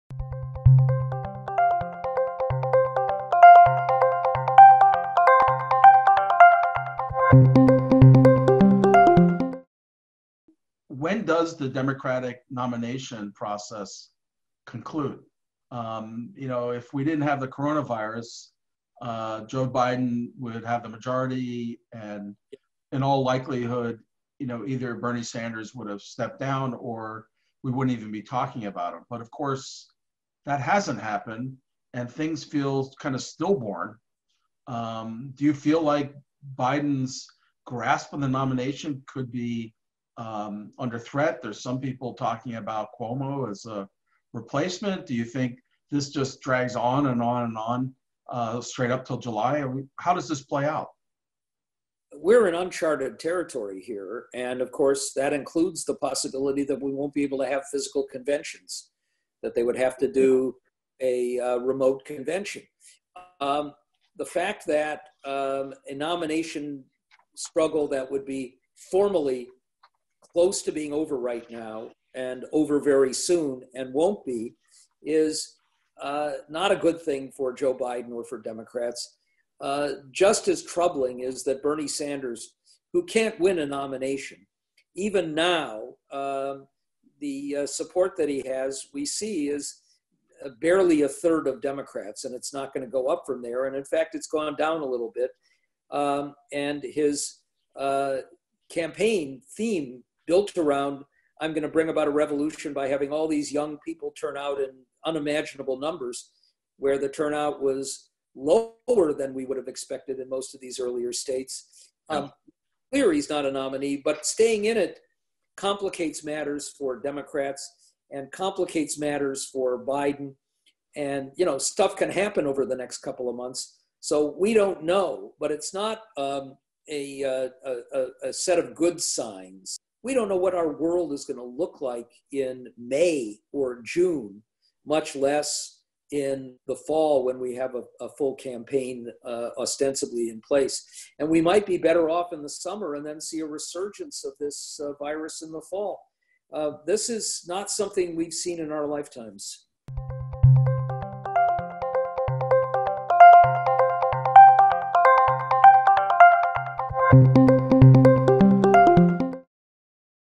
Norm Ornstein and Larry Jacobs on the 2020 Democratic Nomination Process
Excerpt from "Coronavirus and Dysfunction in Washington, D.C."

The Constitutional design of three branches checking each other is broken. Modern presidents have expanded executive power long before Donald Trump's inauguration. But the assumption the Framers made-- that loyalty to one's own institution and its role – no longer applies in an era of tribal loyalties when Republicans in both the House and Senate routinely unify as a block to support President Trump.The result: Trump is asserting new powers or stretching prior precedents to the breaking point. Why has Congress acquiesced and could we witness a resurgence?

This event features scholar of Congress Norm Ornstein and longtime political commentator Professor Larry Jacobs to discuss the latest developments in the coronavirus pandemic and the response in Washington, D.C.